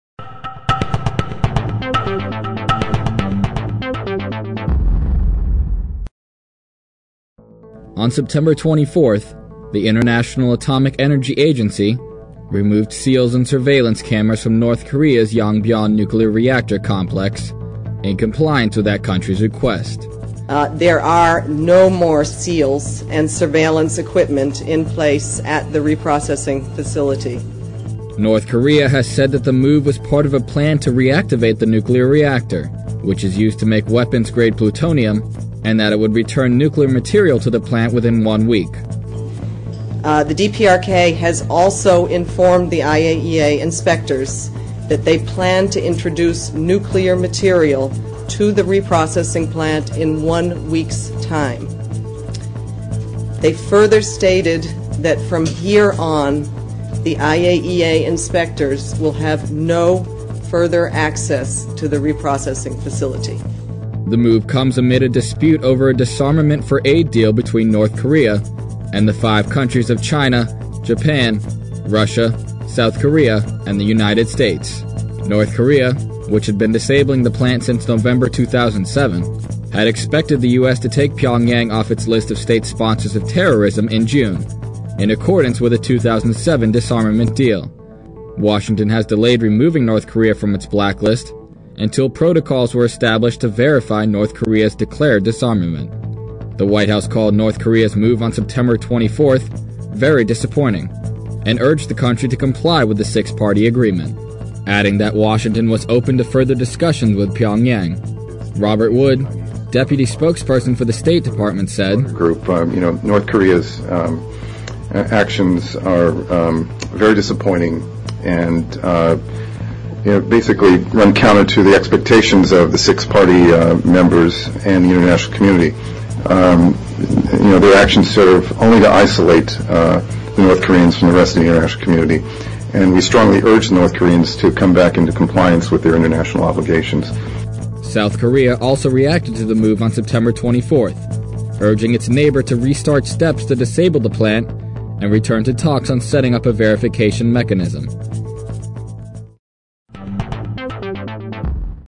IAEA removes seals from North Korea nuclear - IranVNC.com
Washington, October 8 (IranVNC) - On September 24, the International Atomic Energy Agency [IAEA] has removed seals and surveillance cameras from North Koreas Yongbyon nuclear reactor complex in compliance with that countrys request.

Full transcript available at IranVNC.com